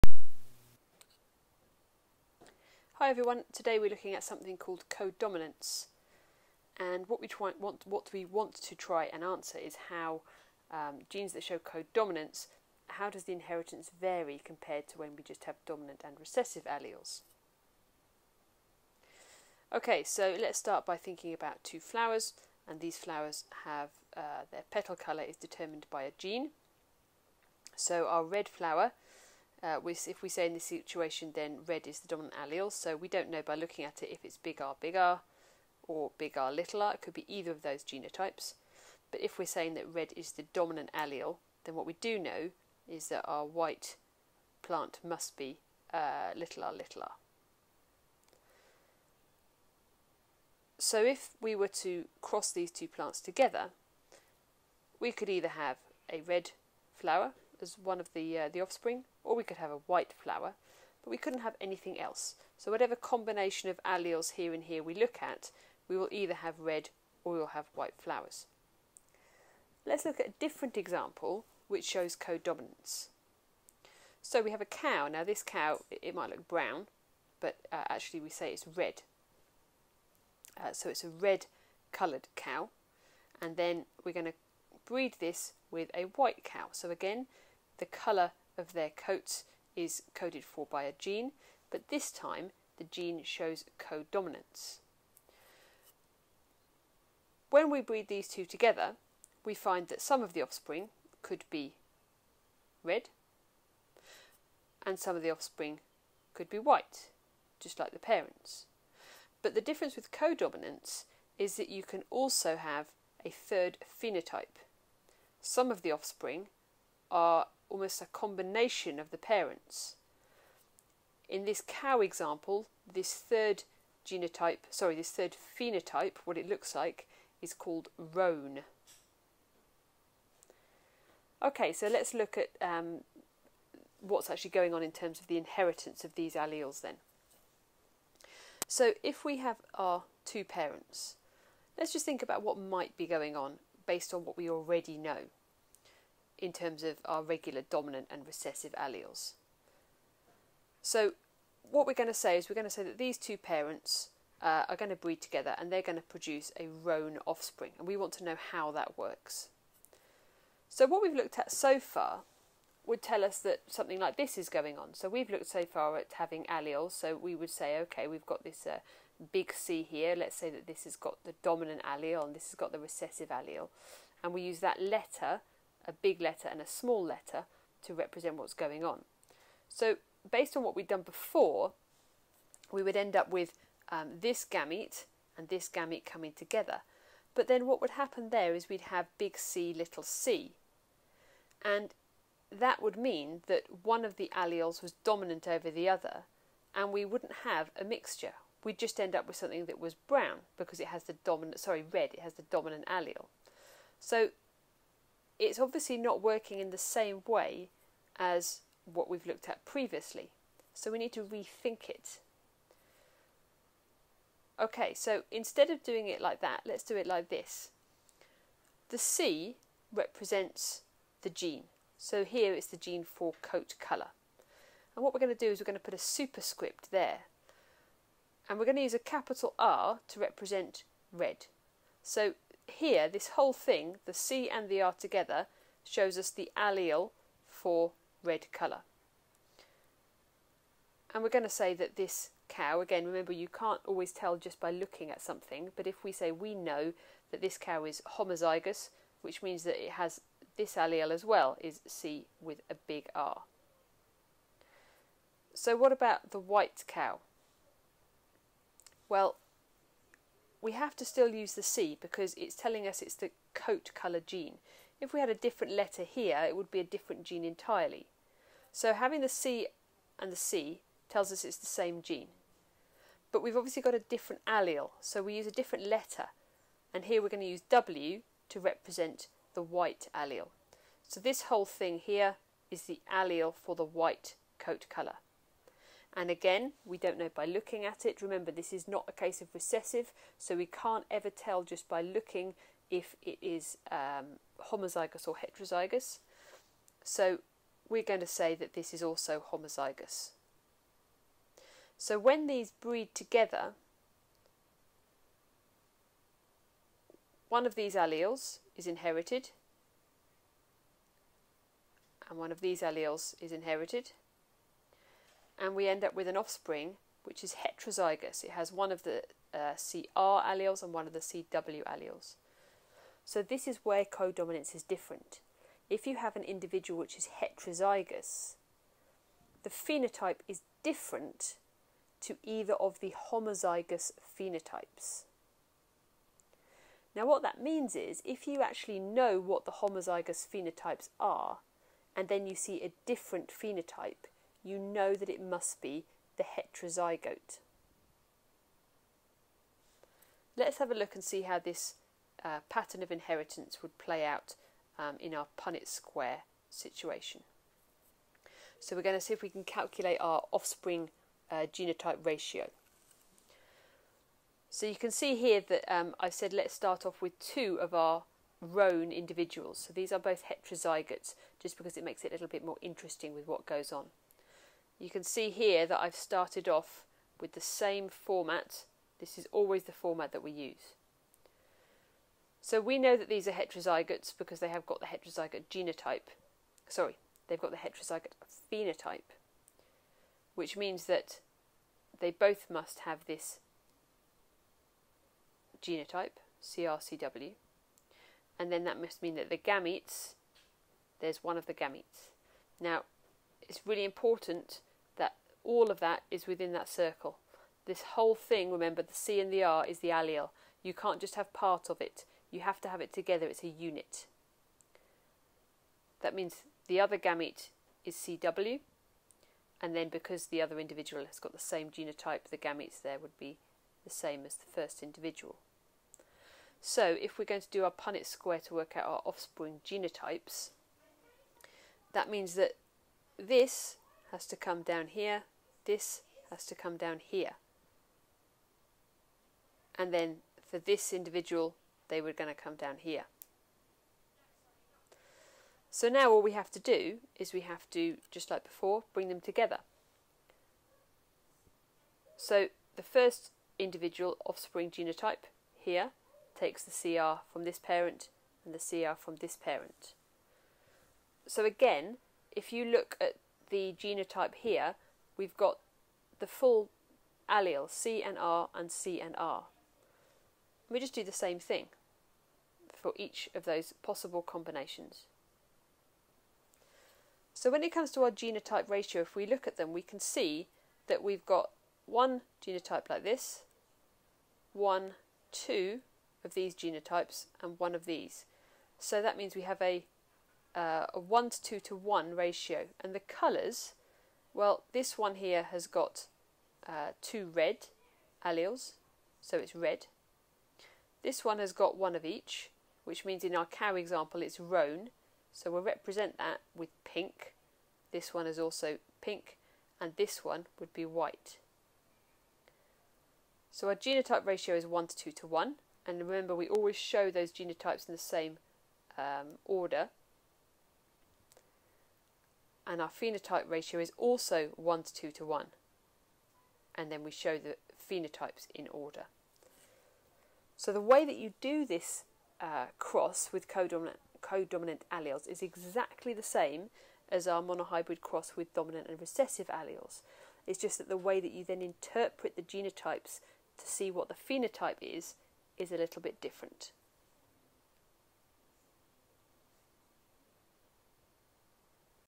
A2 Biology - Codominance
A Level Biology, A2 level. Topic inheritance & Genetics - An explanation of codominance / how codominant genes are inherited.